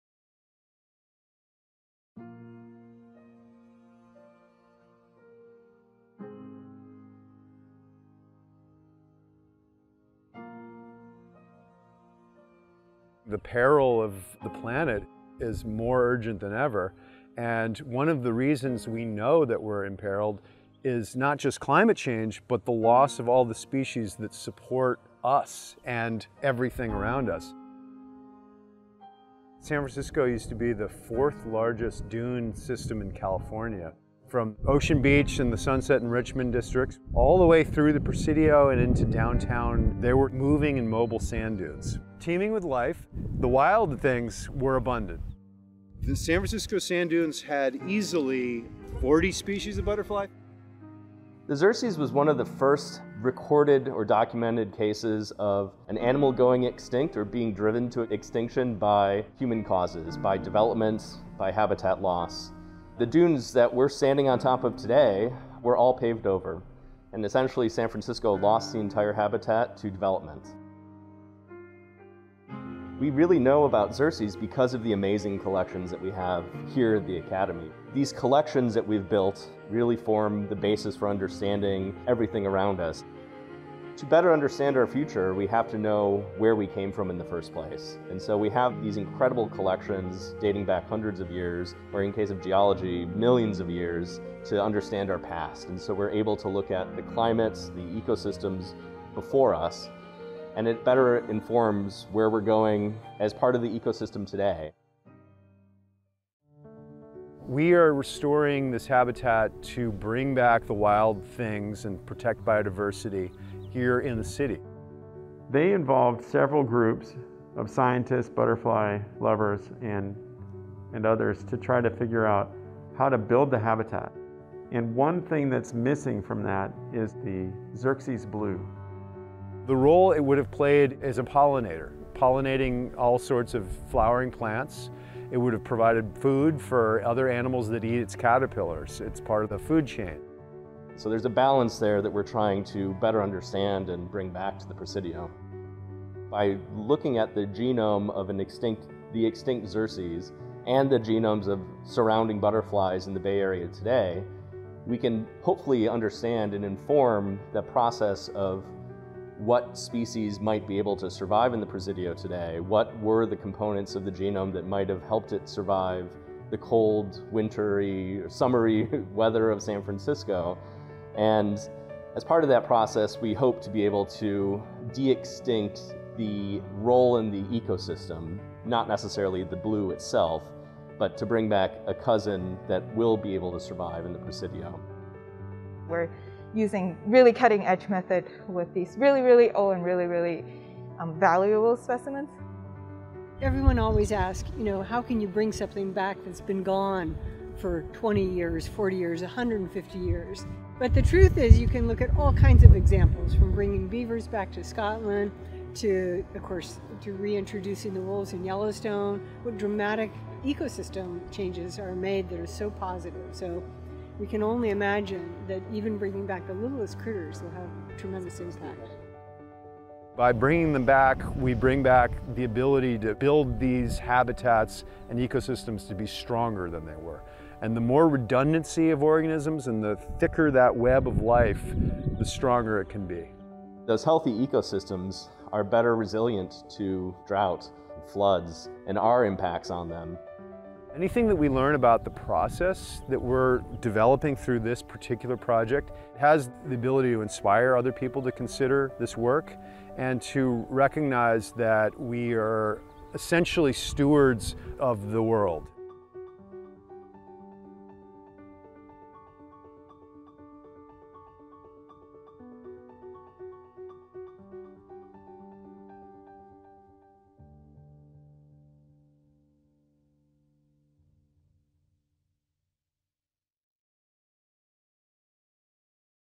Extinction to Stewards: Learning from Xerces Blue
Academy scientists, in collaboration with the Presidio Trust and other stakeholders, are trying to bring back a local butterfly driven to extinction in the 1930s. The butterfly's role in San Francisco's dune habitat is vital to keeping that ecosystem healthy and resilient. The lessons we learn from this project in our backyard could provide information for regenerating other ecosystems across California and beyond.